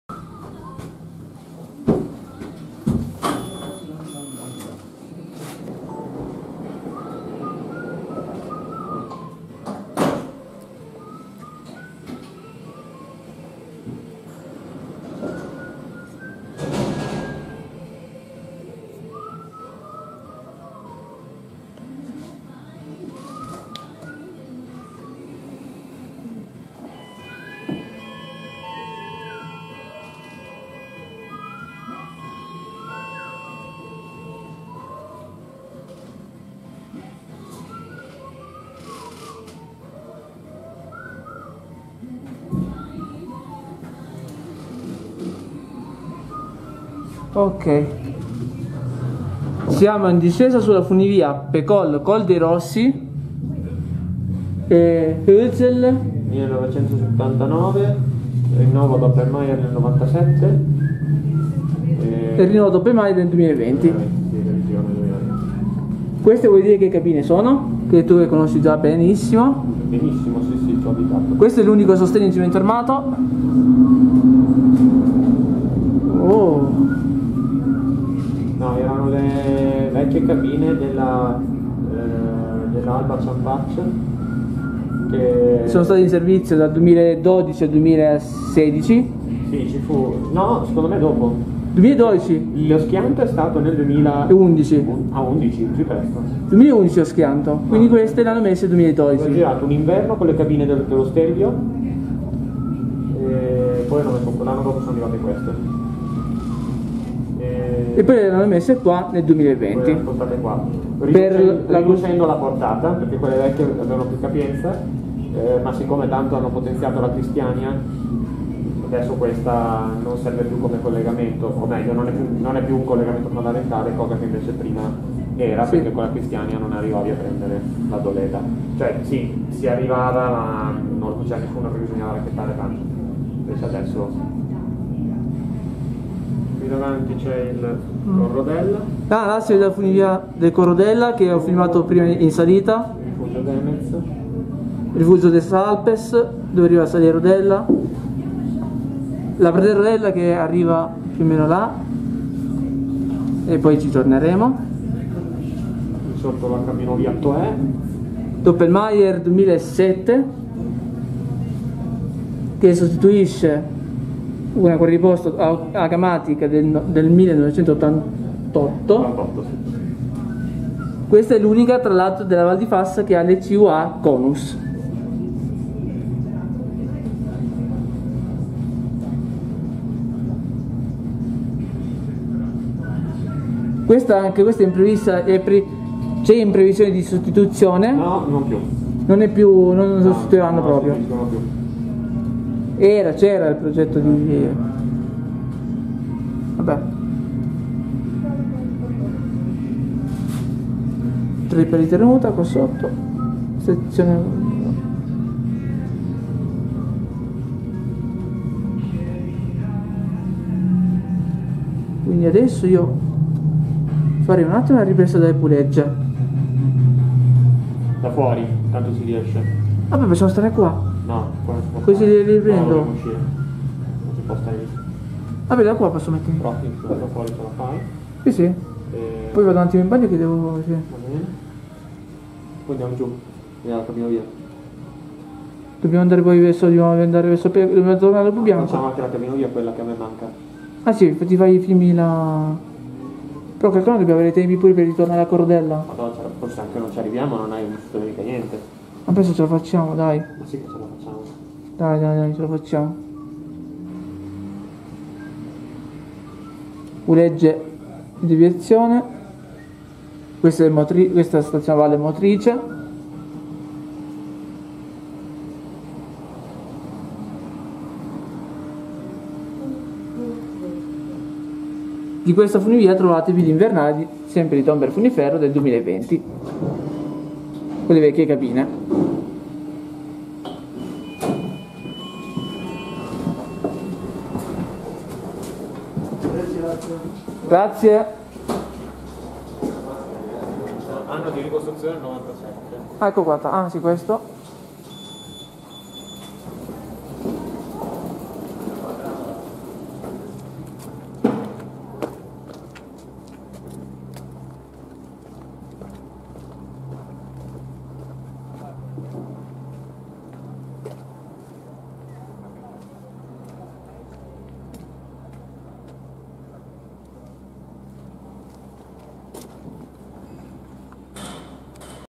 Discesa integrale funivia Hölzl "Pecol - Col dei Rossi" - Canazei (TN)
Funivia (76 - ATW) a va e vieni costruita da Hölzl nel 1979 e ricostruita nel 1997. Nell’estate 2020 è stata revisionata da Doppelmayr montando le Ex cabine della funivia Ciampac ad Alba di Canazei che sono di modello Swoboda; ha un unico sostegno in cemento armato e le tipologie delle stazioni sono valle motrice e monte tenditrice. La funivia è lunga 1308 metri che parte da Pecol e arriva accanto alla nuova seggiovia Kristiania e al Funifor Doleda.